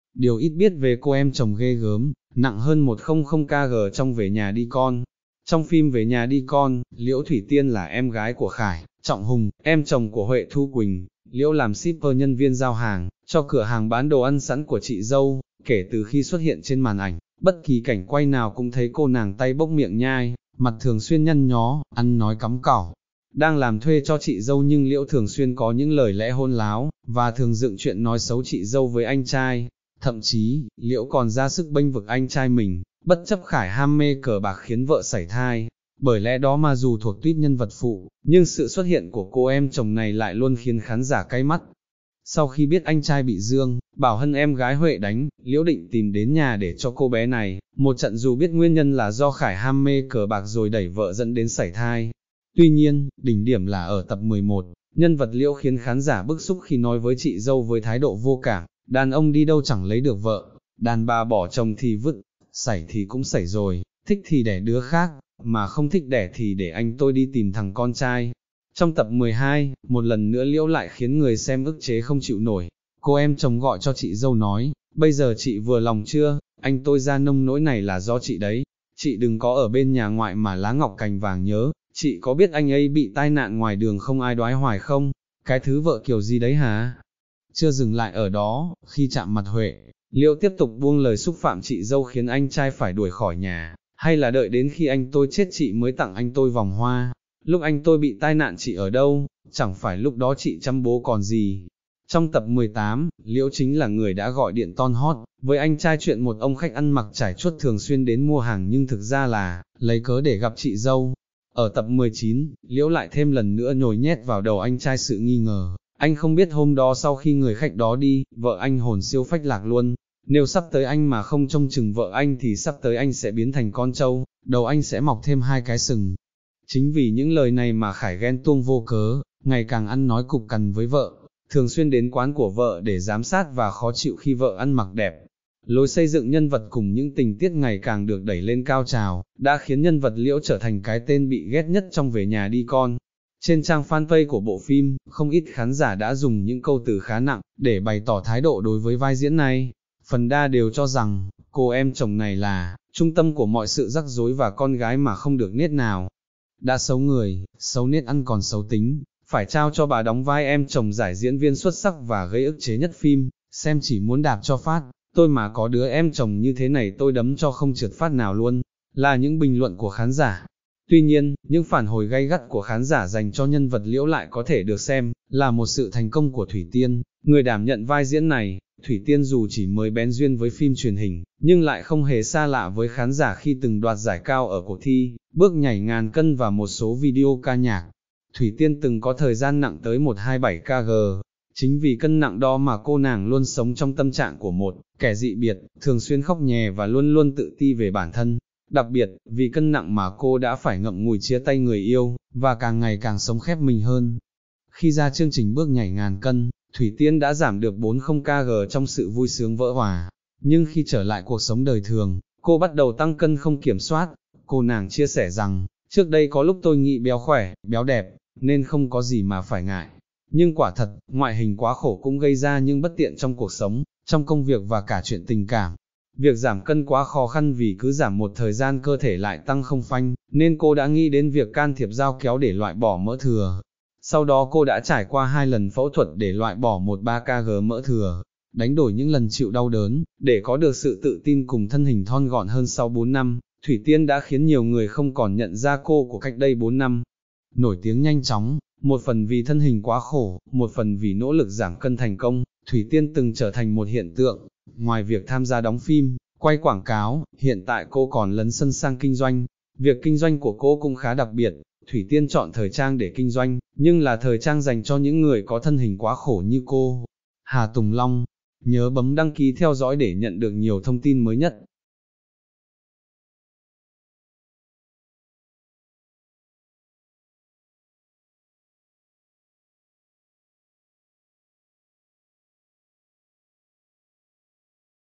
Điều ít biết về cô em chồng ghê gớm, nặng hơn 100kg trong “Về nhà đi con”
(Dân trí) - Dù thuộc tuýp nhân vật phụ nhưng Liễu - cô em chồng vừa “xấu người xấu nết” lại bị ghét nhất bộ phim “Về nhà đi con”. Ít ai biết rằng, ngoài đời, cô em chồng này từng rất khốn khổ vì thân hình quá khổ của mình.